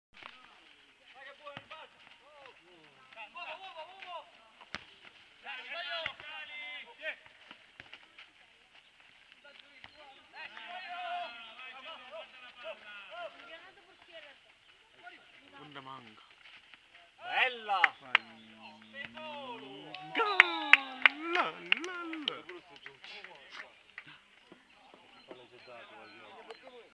Nuove Aquile - Foria San Mauro 1-3 (0-3 M. Ranauro) [28/05/2011]
Partita già chiusa al 40' del primo tempo, quando Mario Ranauro triangola splendidamente con Diego Ranavolo e, lanciato a tu per tu con il portiere, lo batte segnando il suo terzo gol in campionato. In avvio di ripresa i padroni di casa accorciano le distanze, ma il risultato non cambierà più.